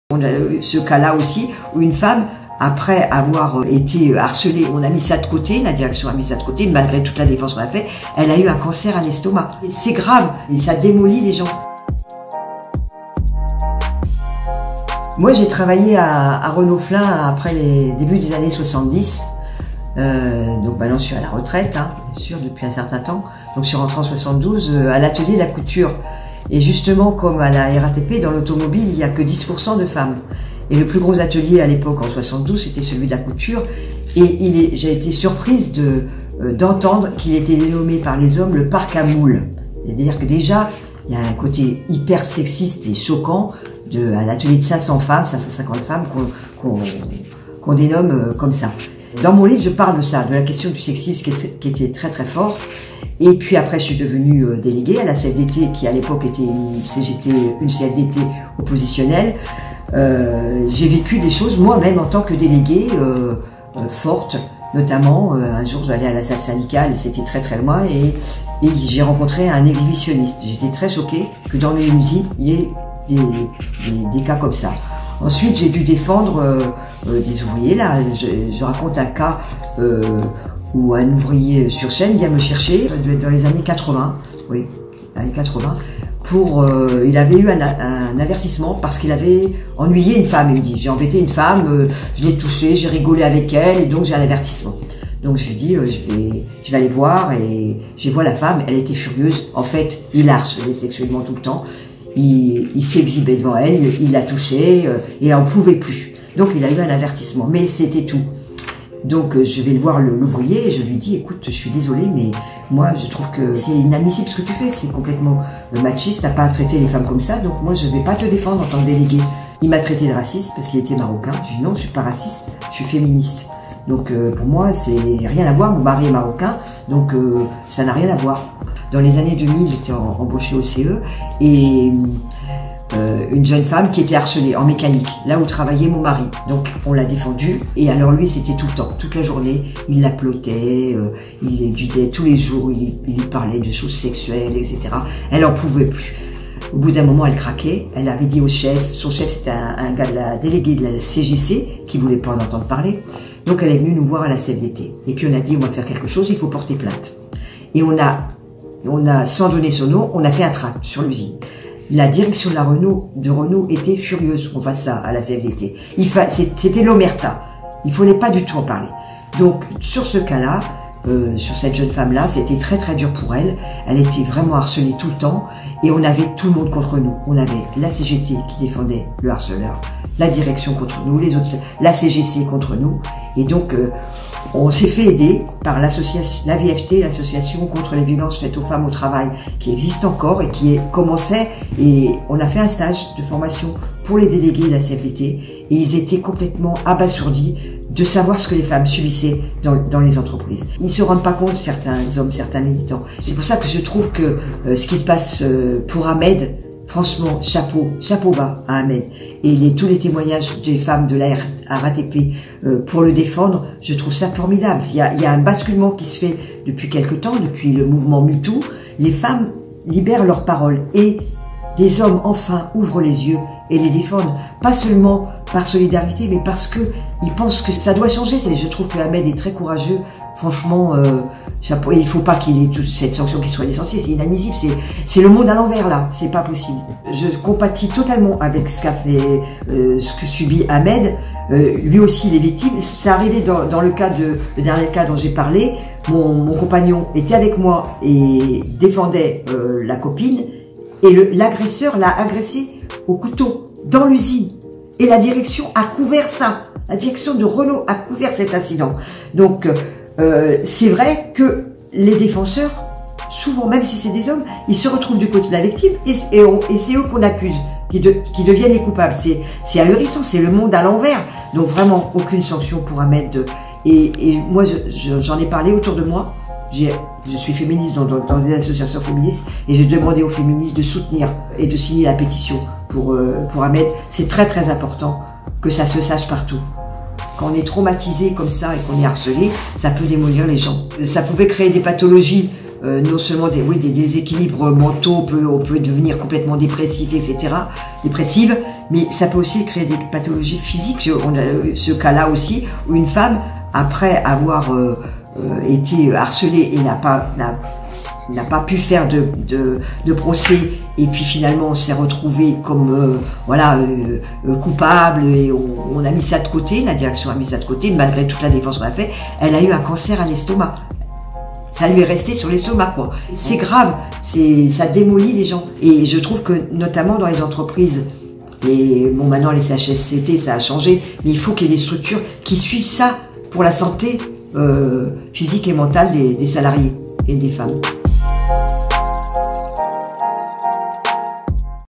Fabienne Lauret: «La parole des femmes se libère, et des hommes enfin les défendent! Chapeau Ahmed!»
Fabienne Lauret, féministe, autrice et ex-ouvrière à Renault Flins, raconte son combat contre le sexisme à l'usine à partir des années 70, et les difficultés à combattre le harcèlement et les violences sexistes, y compris dans les syndicats. Elle apporte son soutien à Ahmed Berrahal : « Les femmes libèrent leur parole, et des hommes enfin ouvrent les yeux et les défendent ! Chapeau à Ahmed ! »